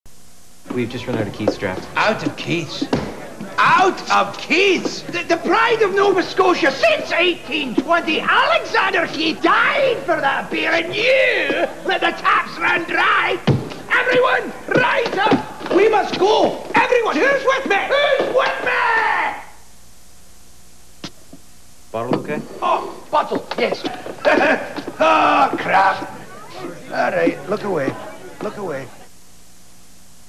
"Who's With Me?" (Alexander Keith's beer ad, circa 2003)
One of many classic Keiths TV commercials starring Robert Norman Smith as the nutty Scot.

(Pulled this out while going through a small stack of videotapes)